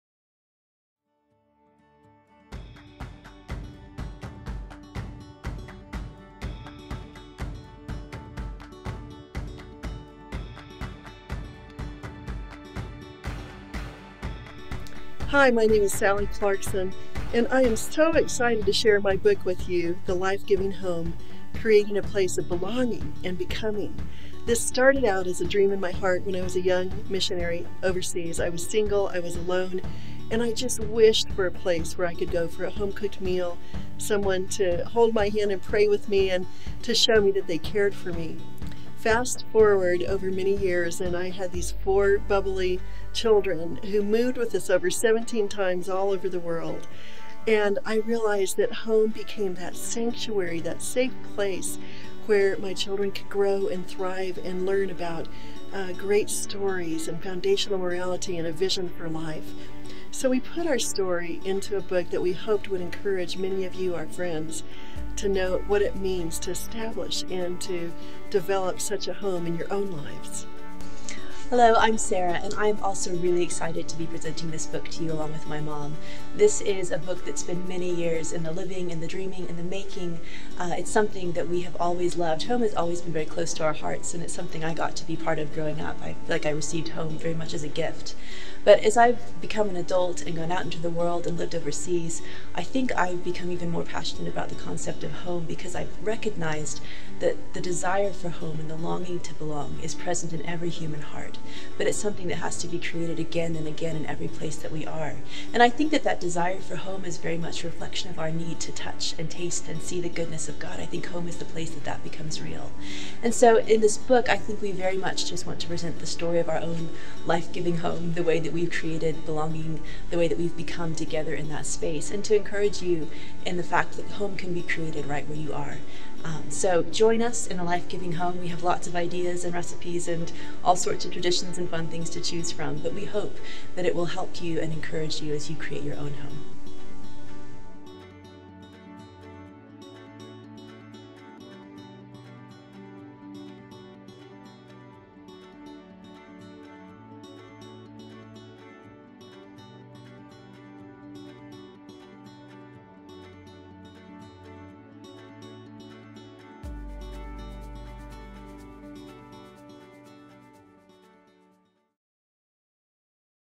"The Lifegiving Home" Book Trailer
Be inspired and encouraged to cultivate a great story in your home, through the upcoming book "The Lifegiving Home," by Sally and Sarah Clarkson. Find out how to make your home a place of belonging and becoming. Preorder now! Official release February 2nd.

Make sure to also pick up your copy of the accompanying bible study guide, "The Lifegiving Home Experience", by Sally and Joel Clarkson. This inspiring book will help you apply the ideas from "The Lifegiving Home" in your own life.